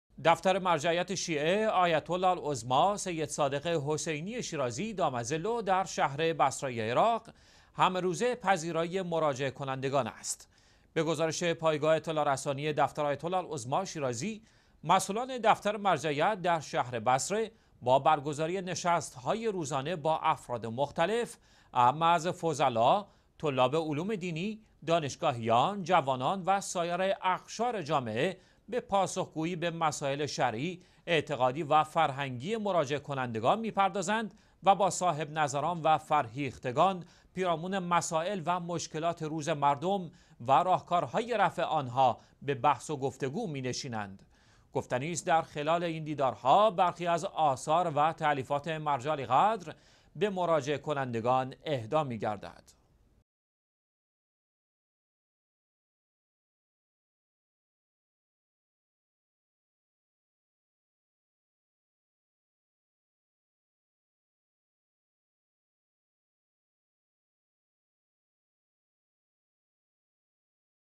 دیدارهای روزانه دفتر مرجعیت شیعه آیت الله العظمی شیرازی، در شهر بصره عراق/۱۶ دی ۱۳۹۵
دیدارهای روزانه دفتر مرجعیت شیعه آیت الله العظمی شیرازی، در شهر بصره عراق

دفتر مرجعیت شیعه آیت الله العظمی سید صادق حسینی شیرازی دام ظله در شهر بصره عراق همه روزه پذیرای مراجعه کنندگان است.

بنابر گزارش پایگاه اطلاع رسانی دفتر آیت_الله_العظمی_شیرازی : مسئولان دفتر مرجعیت در شهر بصره با برگزاری نشست های روزانه با افراد مختلف اعم از فضلا، طلاب علوم دینی، دانشگاهیان، جوانان و سایر اقشار جامعه به پاسخگویی به مسائل شرعی، اعتقادی و فرهنگی مراجعه کنندگان می پردازند و با صاحب نظران و فرهیختگان پیرامون مسائل و مشکلات روز مردم و راهکارهای رفع آن ها به بحث و گفت و گو می نشینند.

گفتنی است در خلال این دیدارها برخی از آثار و تألیفات مرجع عالیقدر به مراجعه کنندگان به دفتر مرجعیت شیعه در بصره اهدا می گردد.

پنج شنبه - ۱۶ دی ۱۳۹۵
الخميس - ٦ ربيع الثاني ١٤٣٨